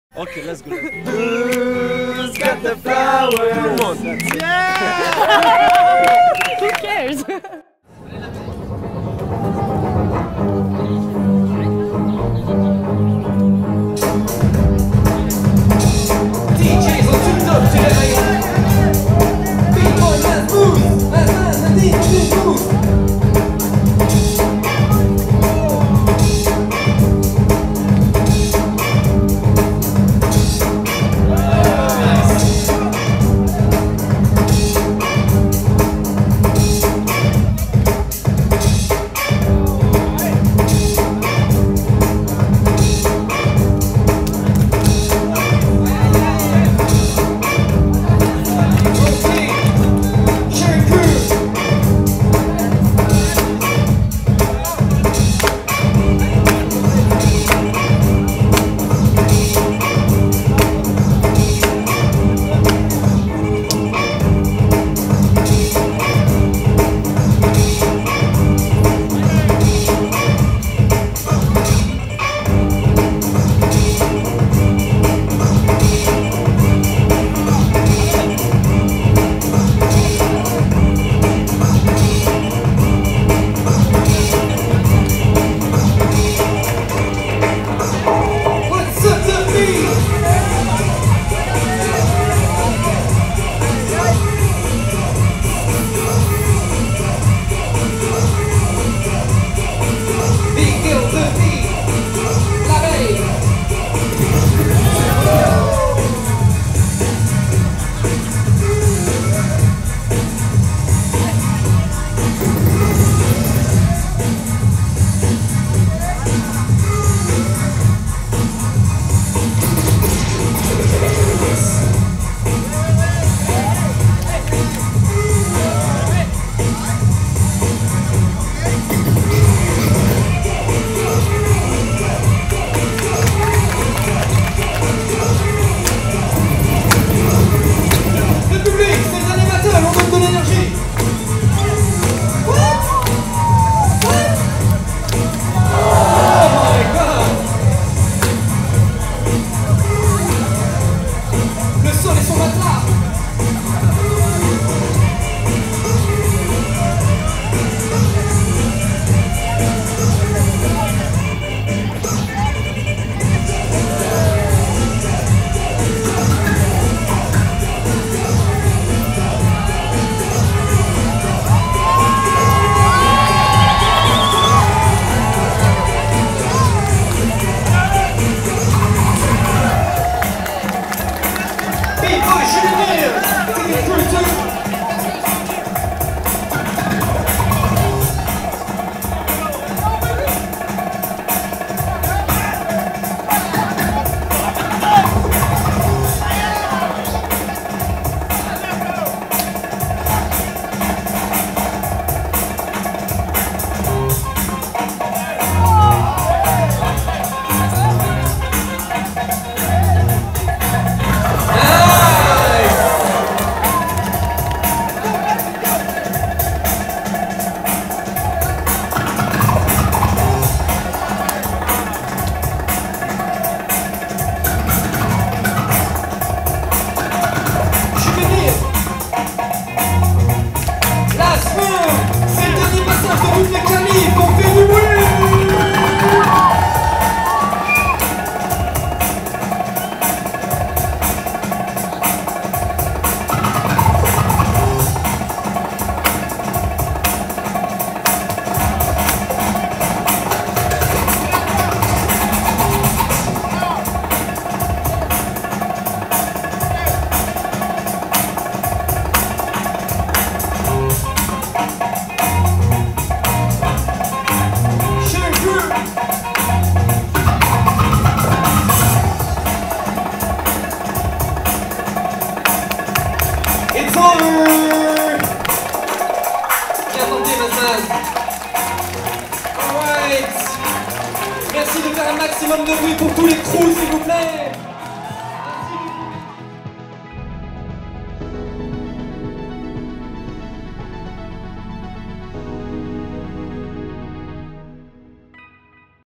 SHARING CREW 🍊 TEKKEN 2 • Prelims 2018
3vs3 Break Prelims at Who got the flower ?! 2018
Sharing Crew vs Tekken 2

Judges:
• Iron Monkey (Renegade Rockers)
• Kate (South Front)
• Tim (Flying Steps)
• LB (Smac 19)
• Sissy (Melting Force)

The Colab • 3vs3 Battle • Junior 1vs1 • 2vs2 Footwork • 1vs1 Toprock